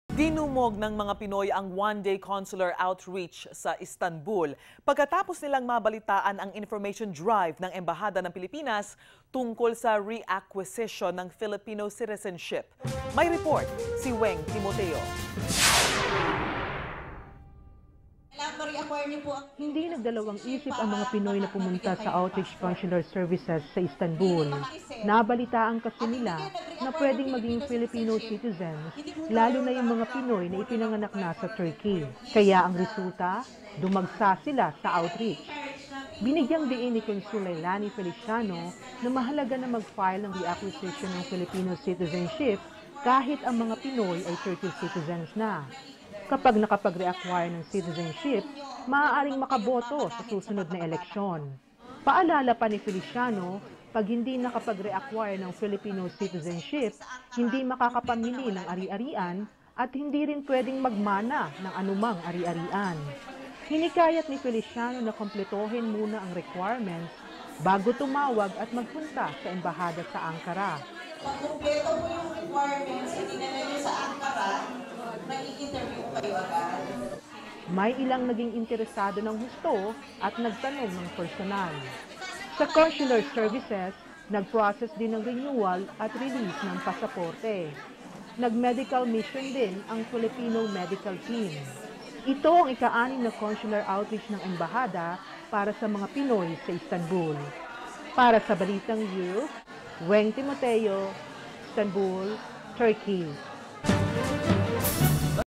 CONSULAR OUTREACH IN ISTANBUL, TURKEY (REACQUISITION OF FILIPINO CITIZENSHIP)
DECEMBER 2013, ISTANBUL -- DINUMOG NG MGA PINOY ANG ONE-DAY CONSULAR OUTREACH SA ISTANBUL PAGKATAPOS NILANG MABALITAAN ANG INFORMATION DRIVE NG EMBAHADA NG PILIPINAS TUNGKOL SA REACQUISITION NG FILIPINO CITIZENSHIP SA ILALIM NG REPUBLIC ACT NO. 9225